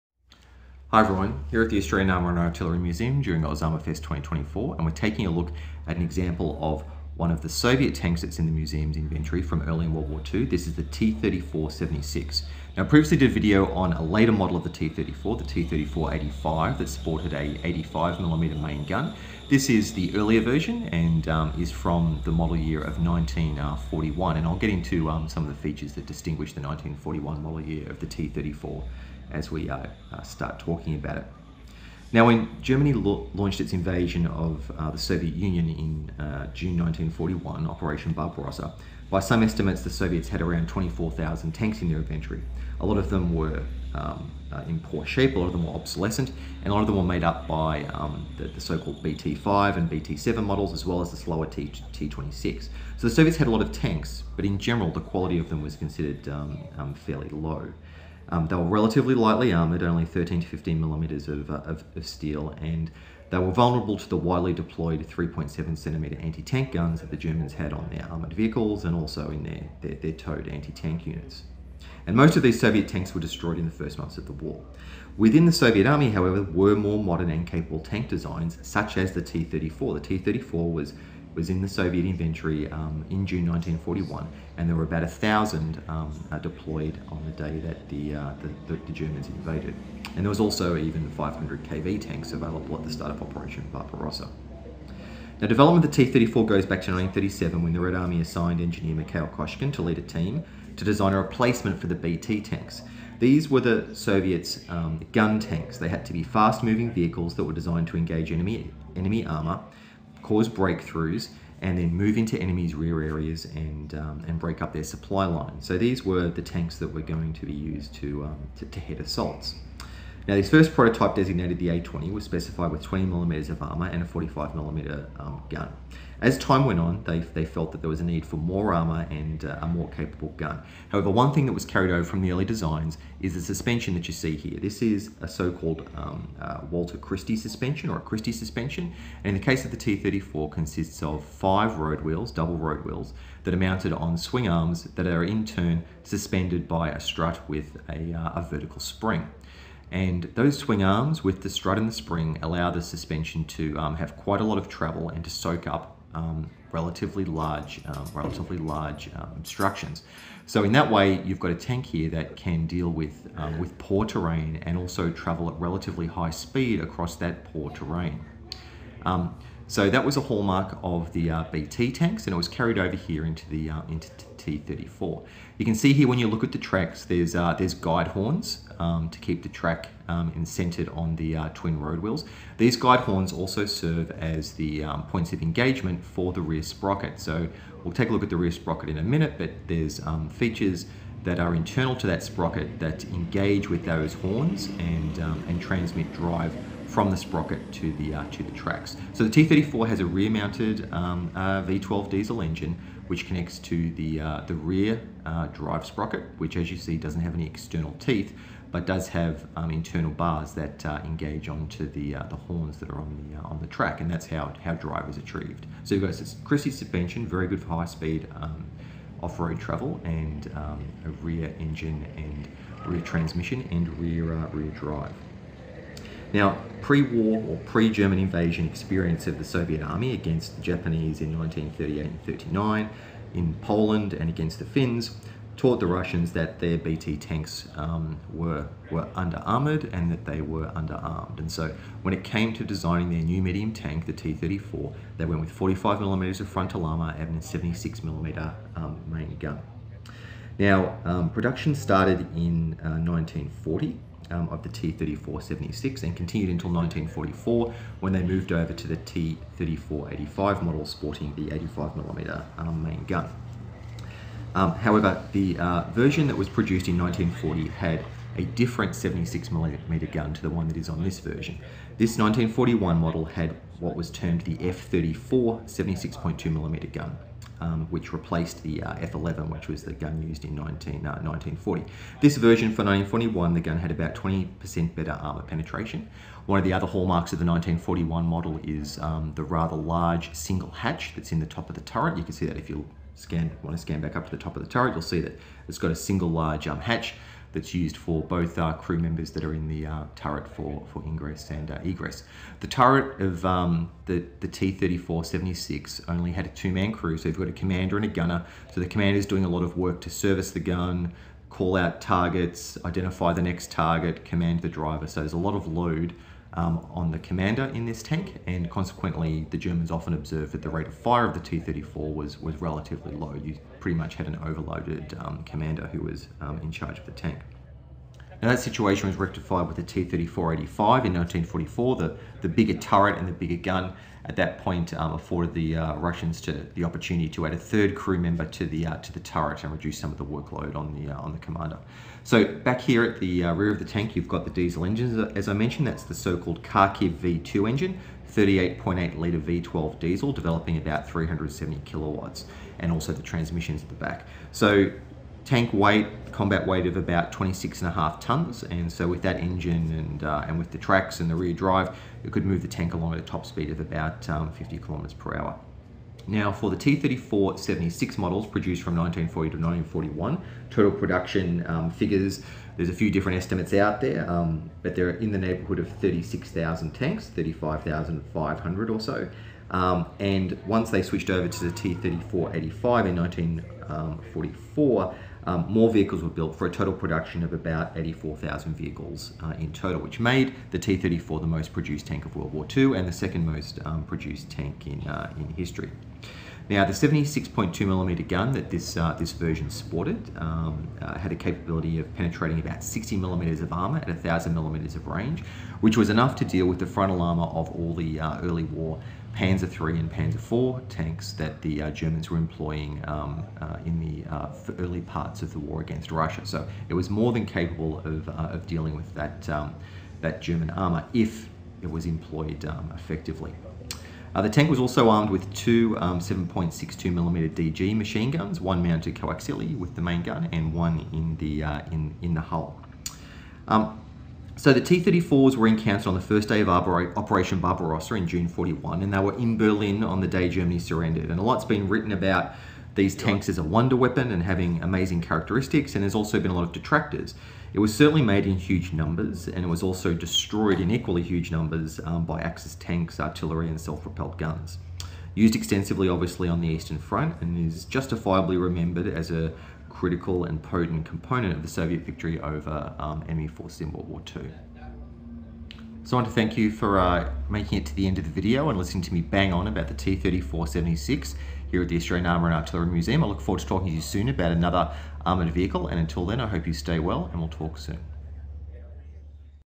T34-76 at AusArmourFest 2024. Soviet Tank.
When Germany launched the invasion of Russia at the end of June 1941, by some estimates the Soviets had about 24,000 tanks in their inventory. Way less than half were ever truly deployable and many were obsolescent. High speed tanks like the BT-5 and BT-7, and the slower T26 were available in their tens of thousands, but were generally very lightly armoured (maximum 13-15mm of steel) and were vulnerable to the widely deployed 3.7cm anti-tank guns of the Germans, that were both towed and vehicle mounted. Most of these Soviet tanks were destroyed in the first months of the war. Within the Soviet army however, were more modern and capable tank designs, and in June 1941 these were in the field, with almost 1,000 T-34s, like the one you see before you, and over 500 KV tanks available.

In 1937, the Red Army had assigned engineer Mikhail Koshkin to lead a team to design a replacement for the BT tanks. This needed to be a fast moving vehicle designed to engage enemy tanks and cause havoc in the enemies rear areas. The prototype, designated A-20, was specified with 20mm of armour and a 45mm gun. It could run either on tank tracks or via power to its road wheels, in line with the high-speed suspension concept that had been used on the older BT tanks. This suspension had been developed by American Engineer Walter Christie and resulted in tanks with high speed, great cross-country performance, and the ability to travel long distances without wearing out tracks. Ultimately, though, the ability to travel without tracks was removed from the design as better track designs emerged, though the rest of the Christie-based suspension features were retained.

Following border skirmishes in the Far East against  Japanese forces in Manchuria in 1938 and 1939, the Russians learned that their existing tanks were deficient in a number of areas. Their armour was too thin, their guns too small, their manufacturing quality was poor, and their petrol powertrains were susceptible to fire. Koshkin built further prototypes with these learnings, ultimately resulting in the T-34s balanced design.

The T34-76 variant was produced from 1940 through to 1944, when it was replaced in production by the T34-85. This newer model mounted a considerably larger turret housing a 85mm ZiS-S-53 gun. Different sources give vary on the production figures for the T-34, but according to the book T-34: Mythical Weapon a total of 35,595 were produced with the 76.2mm gun in seven different factories. Adding in T34-85 variants, total production of the T34 was over 84,000 vehicles. The relocated Kharkov Locomotive plant, UTZ that was moved to Nizhniy Tagil in the Urals after Germany captured the Ukraine, produced almost 29,000 T-34s alone. These figures made it most produced tank of WW2 and the second most produced in history.

The 76.2mm F-34 gun used on the T-34-76 from 1941 onwards was used in a modified form on the KV-1 heavy tank also. Compared to the F11 gun that was mounted in the 1940 model of the T-34, it had about 20% better armour penetration. Reportedly the F34 could penetrate 60mm of armour at 1,000m range, enough to deal with the frontal armour of early war Panzer III & IV tanks. Effectiveness of the gun was limited by the turret only accommodating two crew members, meaning the commander needed to lead the crew, identify targets, and serve the gun. This means commander situational awareness was lower and his ability to identify the next target and direct the driver suffered. This was rectified in the T-34-85 where a third crew member was added to the turret. The T-34 was also armed with two 7.62mm DG machine guns, mounted coaxially and in the hull.

The 1941 model had frontal and side hull armour of up to 45mm, turret was 52-60 mm, hull top was 20mm. Armour levels and quality varied by plant so figures here are general. Lots has been written about the quality of T-34 armour in terms of being excessively hardened and consequently very brittle, and also being adversely affected by poor welding. On the other side, the armour was sloped at up to 60 degrees, making its effective thickness much higher.

The engine used was a 38.8L V12 diesel engine (the Kharkiv V-2 engine) developing 370 kW. Transmission was mounted behind the engine at the at the rear of the vehicle and the drive wheels were similarly at the rear. Chassis was the Christie style suspension with 5 double road wheels per side mounted on pivoting arms. Motion of the arms was controlled by coil springs and coaxial dampers. Tracks were 500-550mm depending on type (both earlier plate and improved waffle pattern links were used on the 1941 model). The setup could move the 26.5t vehicle at a top speed of over 50kph.

T-34s were encountered on the first day of Operation Barbarossa, and were in Berlin on the day Germany surrendered. It was used extensively on the Eastern front and is justifiably remembered as a critical and potent component of the Soviet victory over Axis forces in WW2.